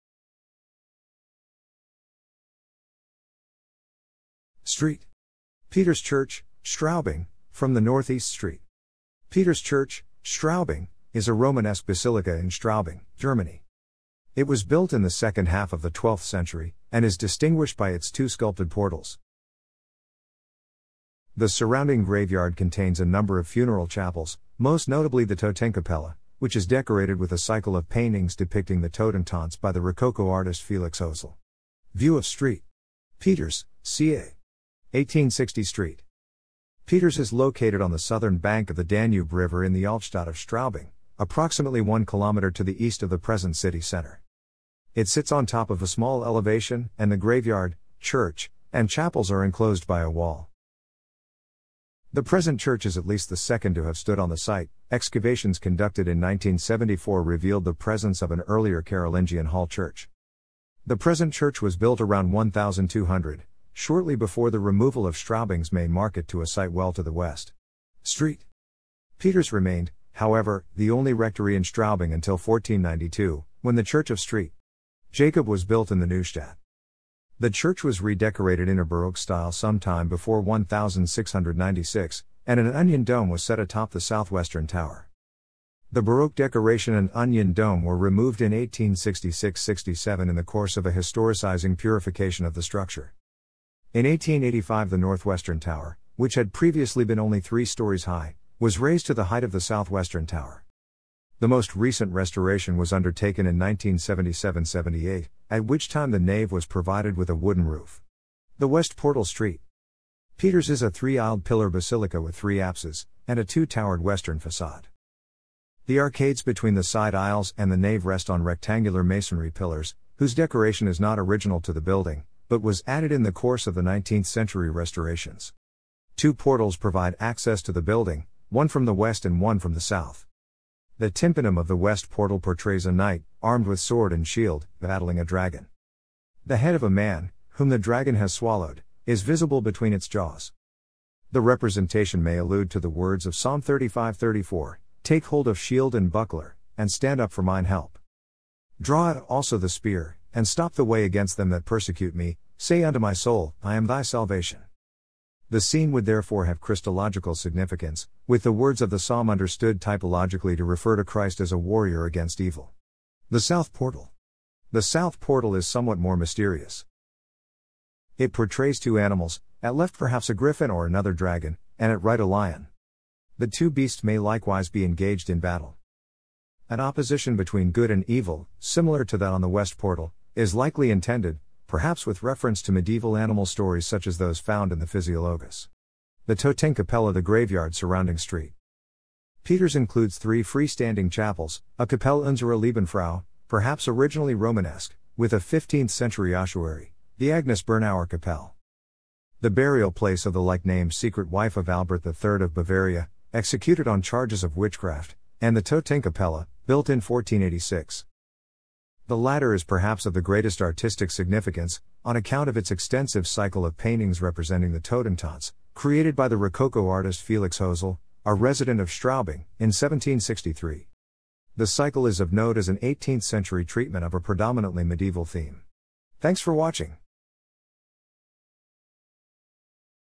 St. Peter's Church, Straubing
St.
Peter's Church, Straubing, from the north east St.
Peter's Church, Straubing, is a Romanesque basilica in Straubing, Germany.
It was built in the second half of the 12th century, and is distinguished by its two sculpted portals.
The surrounding graveyard contains a number of funeral chapels, most notably the Totenkapelle, which is decorated with a cycle of paintings depicting the Totentanz （Dance of the Dead） by the Rococo artist Felix Hözl.
View of St.
Peter's, ca.
1860 St.
Peter's is located on the southern bank of the Danube river in the Altstadt of Straubing, approximately one kilometer to the east of the present city center.
It sits on top of a small elevation, and the graveyard, church, and chapels are enclosed by a wall.
The present church is at least the second to have stood on the site: excavations conducted in 1974 revealed the presence of an earlier Carolingian hall church.
The present church was built around 1200, shortly before the removal of Straubing's main market to a site well to the west （the present-day Neustadt）.
St.
Peter's remained, however, the only rectory in Straubing until 1492, when the Church of St.
Jakob was built in the Neustadt.
The church was redecorated in a Baroque style sometime before 1696, and an onion-dome was set atop the south-western tower.
The Baroque decoration and onion dome were removed in 1866/67 in the course of a historicizing "purification" of the structure.
In 1885 the north-western tower, which had previously been only three storeys high, was raised to the height of the south-western tower.
The most recent restoration was undertaken in 1977/78, at which time the nave was provided with a wooden roof.
The west portal St.
Peter's is a three-aisled pillar basilica with three apses, and a two-towered western facade.
The arcades between the side aisles and the nave rest on rectangula...